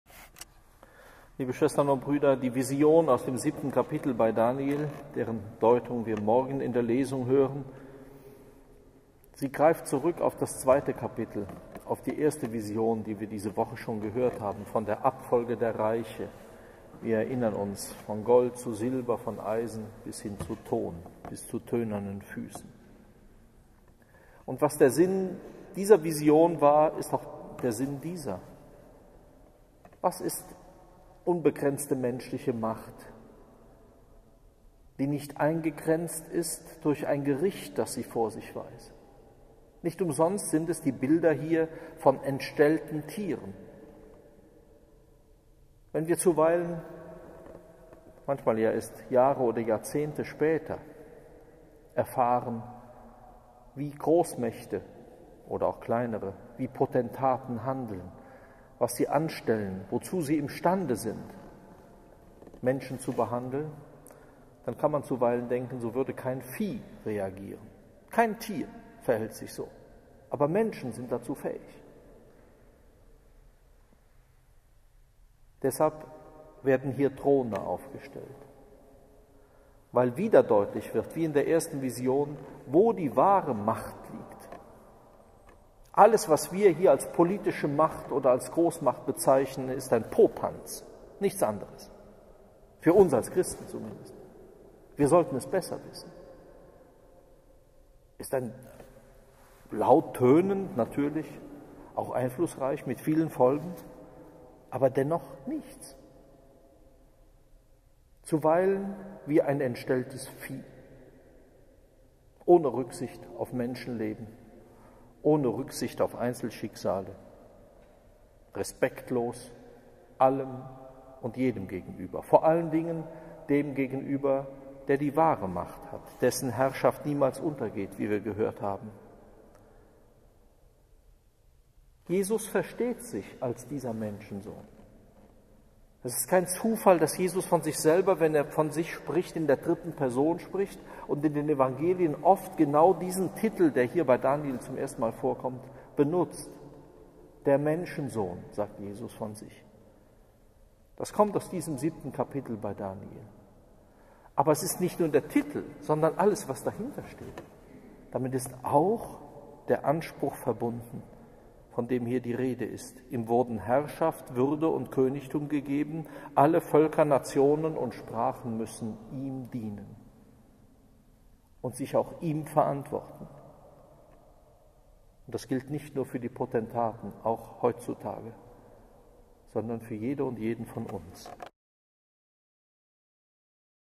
Predigt
Predigt am Fr, 34.Woche von Rektor Dr. Johannes J.Kreier im Kloster am Rande der Stadt/ Saarbrücken/ 2015.